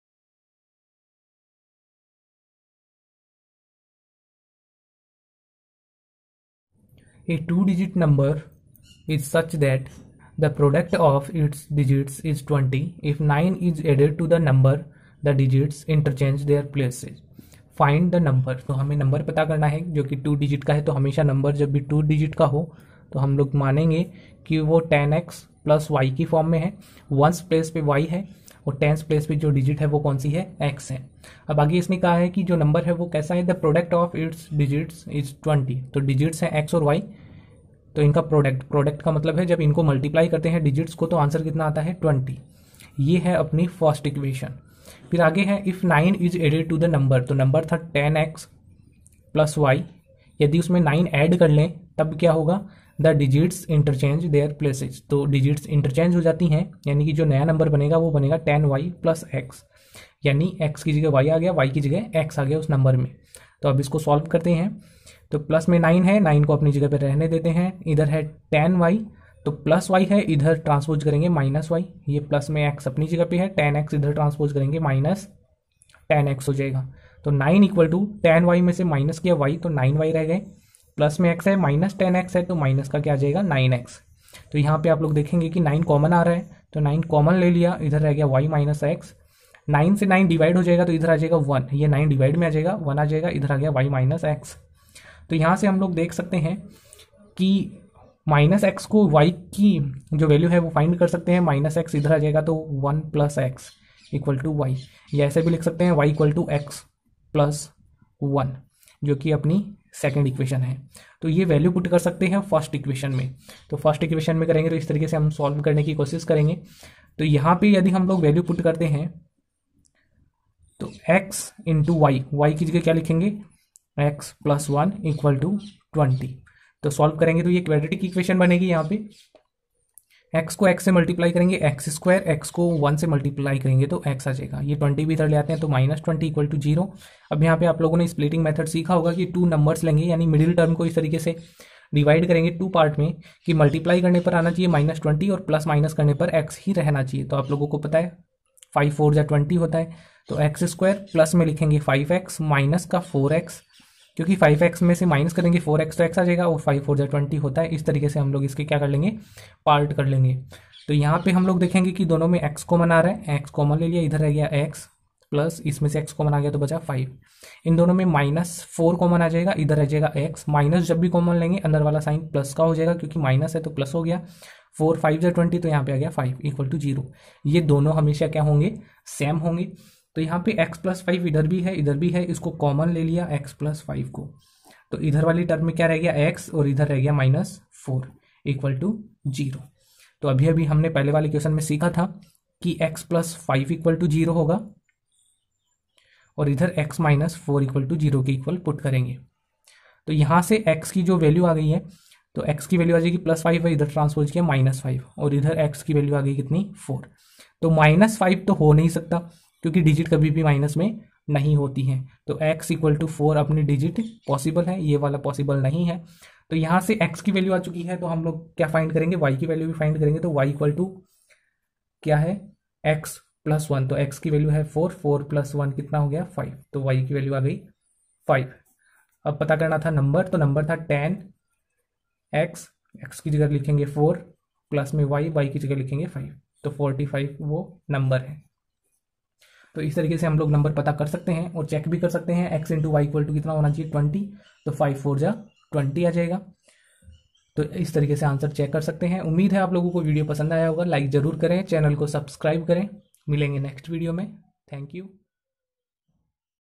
A two-digit number is such that the product of its digits is 20...|| Q.12 Ex.3.7 RD Class 10
Hello friends
In this video we learn to solve question 12 Exercise 3.7 rd sharma book class 10.

Question:
a two digit number is such that the product of its digits is 20. If 9 is added to the number, the digits interchange their places. Find the number.

If you enjoy this video then 👍👍